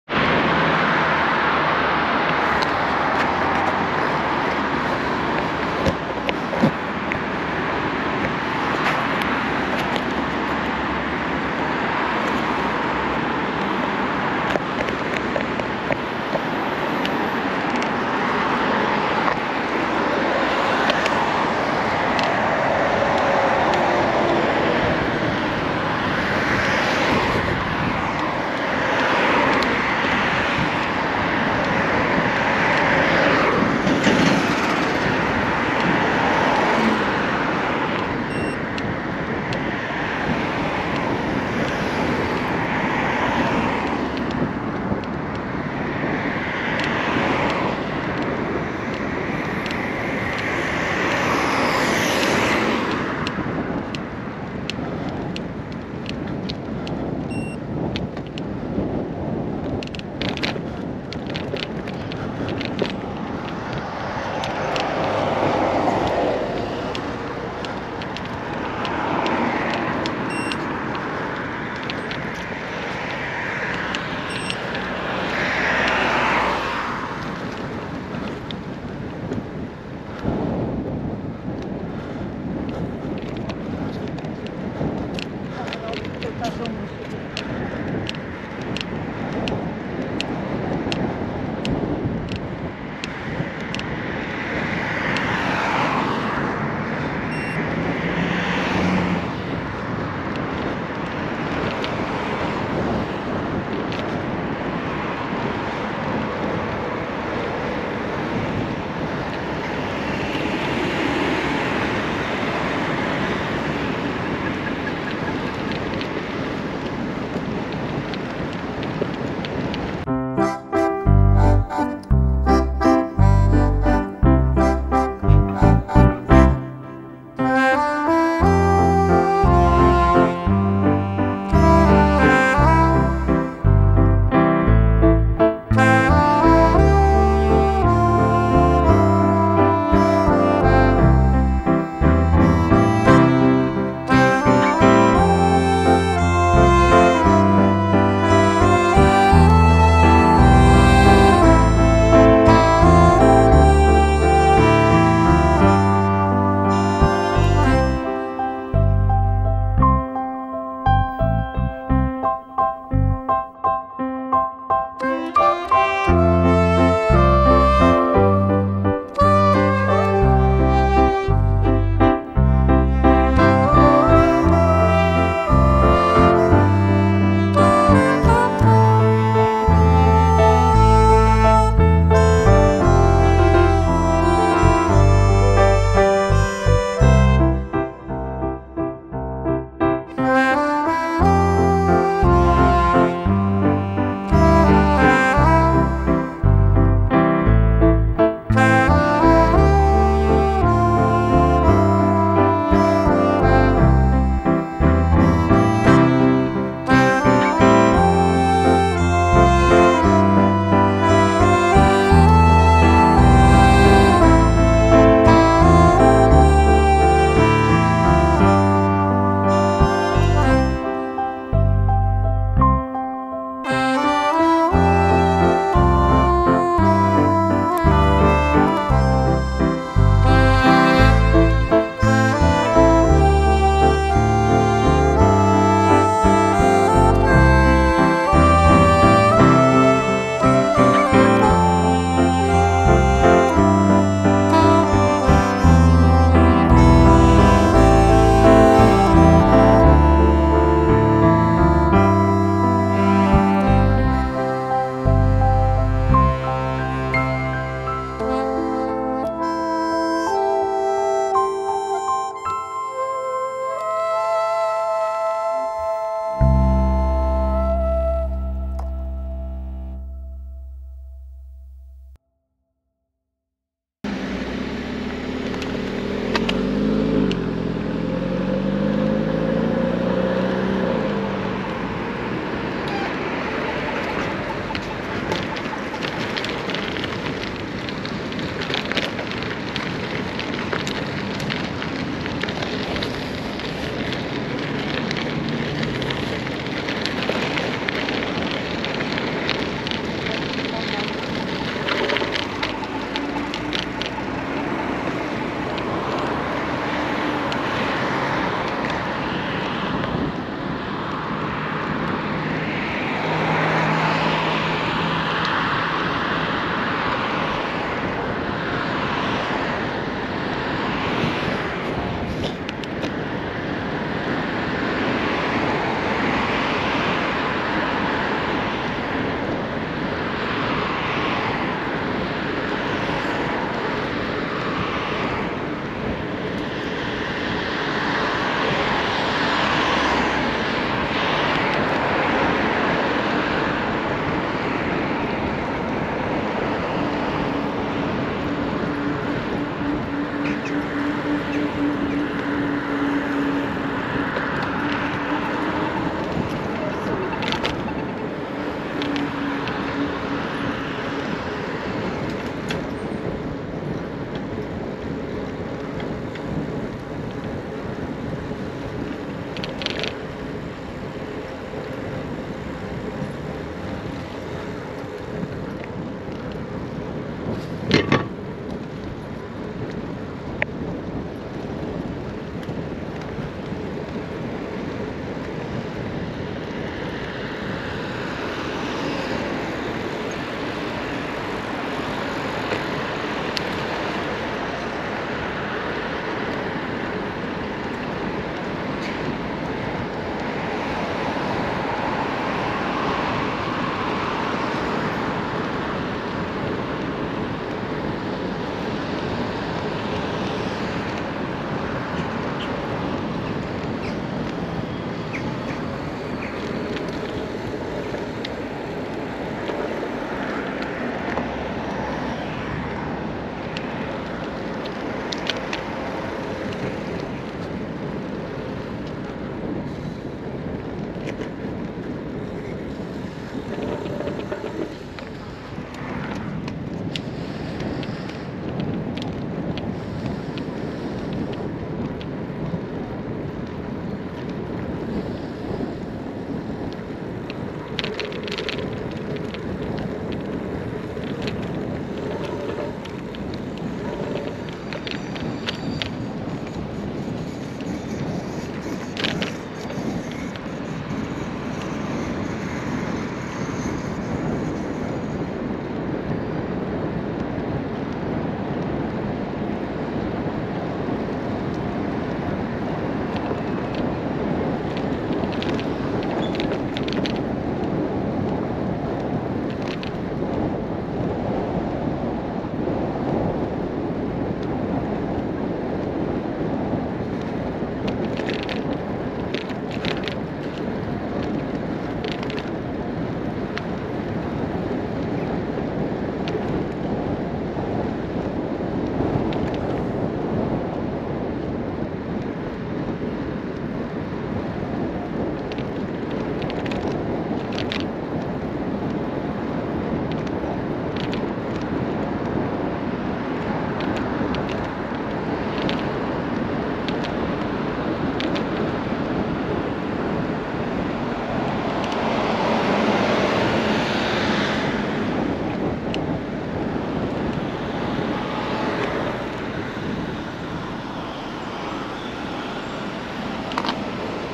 Szwederowo Bydgoszcz: po lewej Castorama grzejniki stalowe c22 300m KFC po czym Lidl Centrum Nurkowe
Osiedle Szwederowo  - Bydgoszcz i okolice na rowerze - mniej znane miejsca